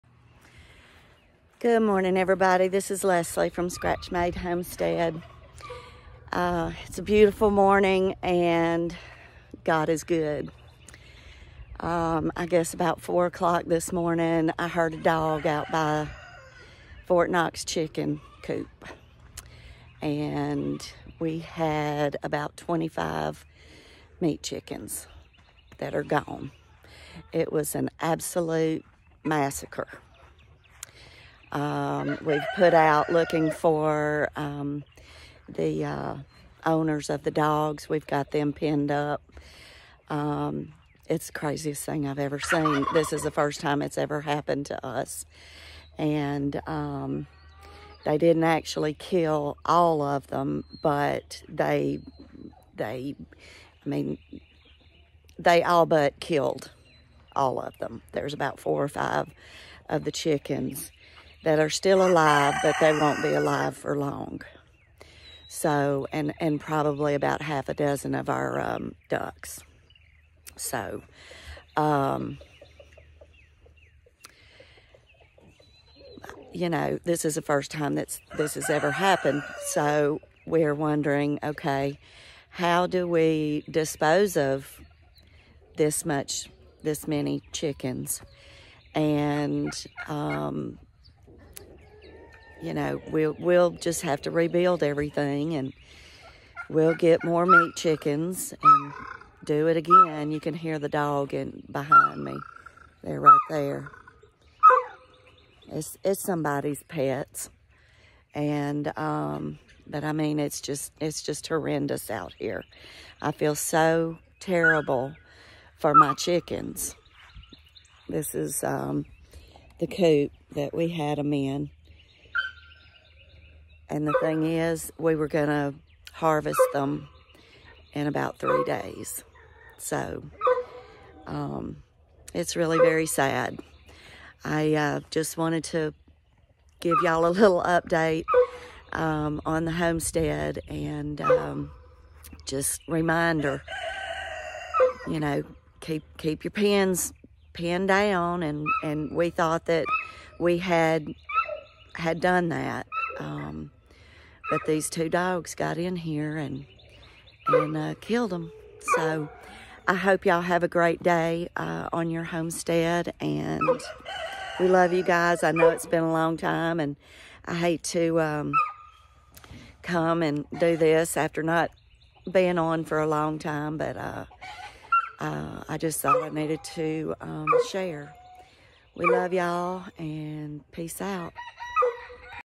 Chickens and dogs don’t mix
Dogs got into our meat chickens and duck pin. It is not good.